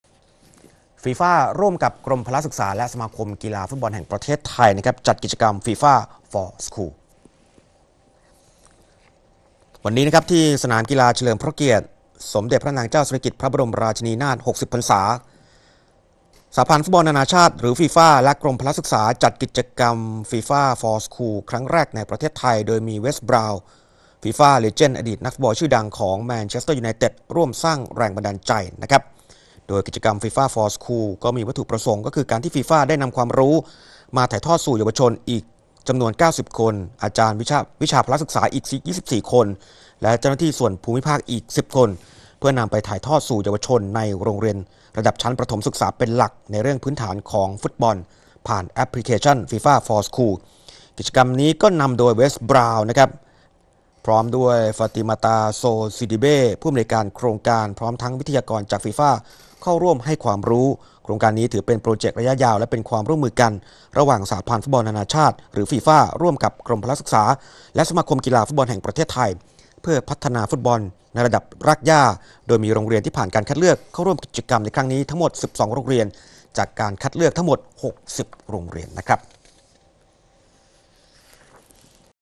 ข่าวกีฬา ฟีฟ่า - กรมพลฯ - ส.ฟุตบอลฯ จัดกิจกรรม FIFA For School ข่าวค่ำ วันที่ 22 มกราคม 2566
ฟีฟ่า ร่วมกับ กรมพลศึกษาและสมาคมฟุตบอล  จัดกิจกรรม FIFA For School